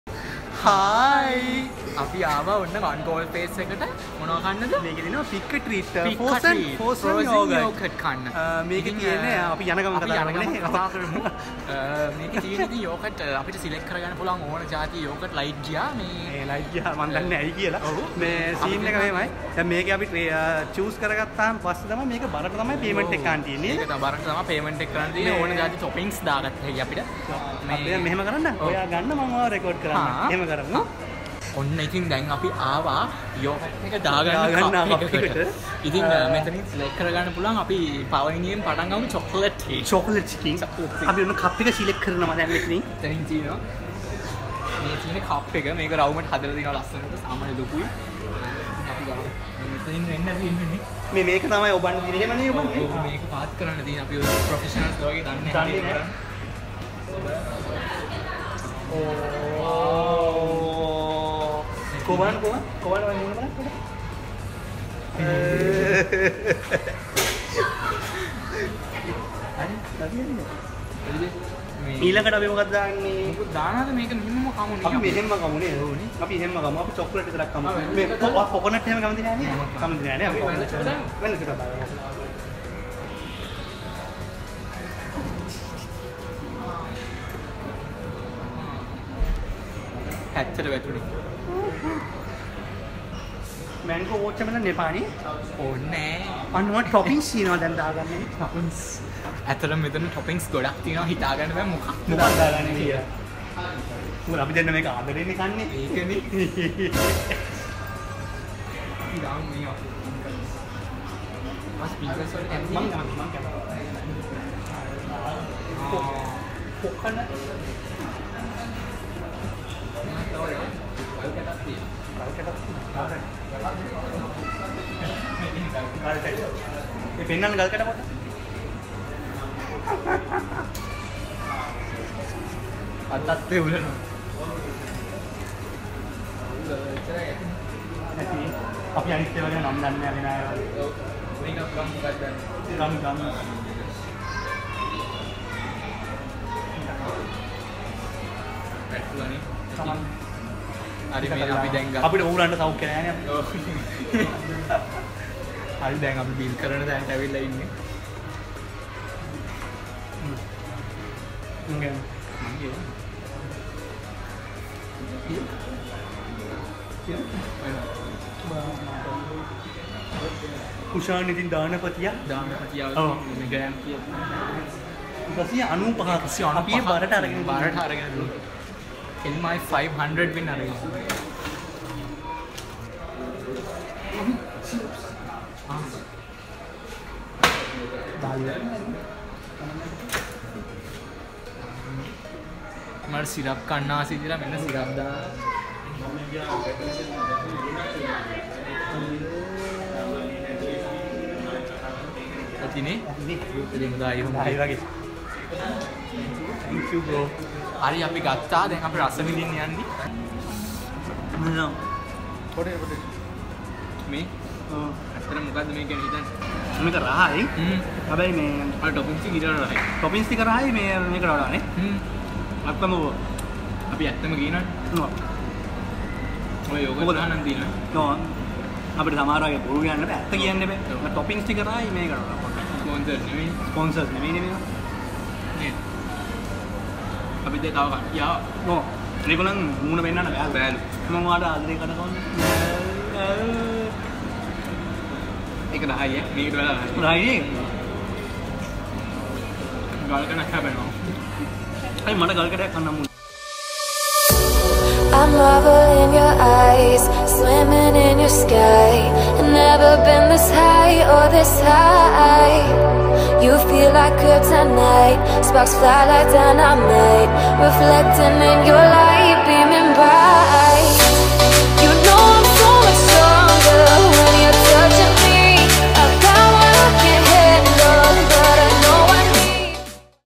ඔන්න අපිත් ගියා One Galle Face එකේ Frozen Yogurt කන්න
A video by Kushan Abeysinghe.